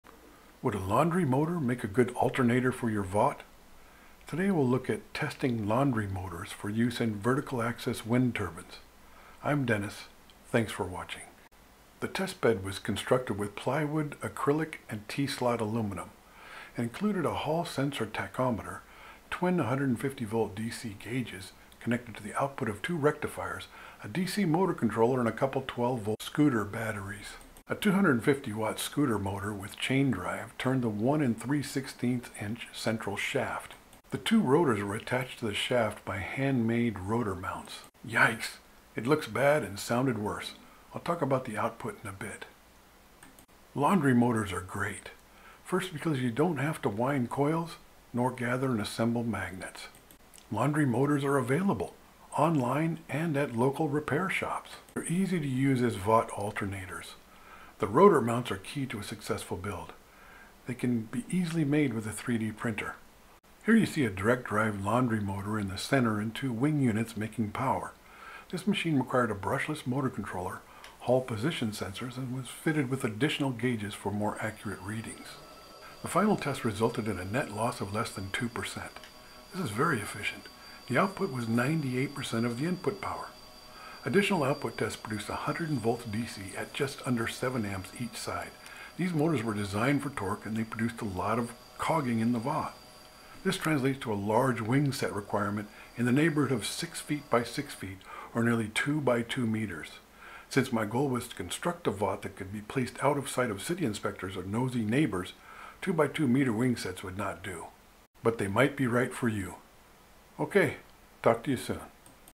LaundryMotorVAWTtests
I tested Laundry Motors for several months to determine if I could use them as VAWT alternators. These are very good motors. With the right sized VAWT these alternators can produce plenty of power.